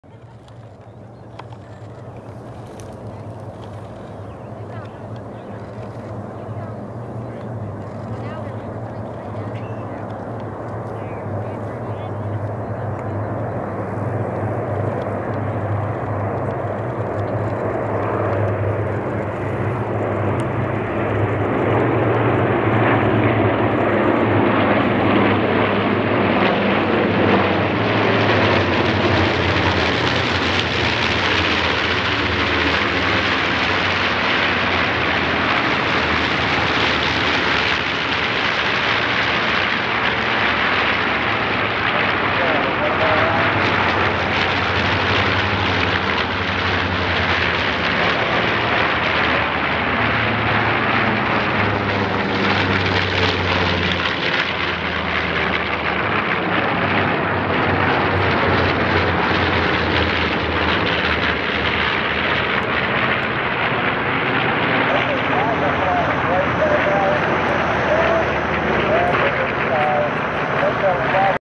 B-25 Flyover of Wright-Patterson Air Force Base
North American B-25 Mitchells fly over Wright Patterson Air Force Base for the 70th anniversary of the Doolittle Raid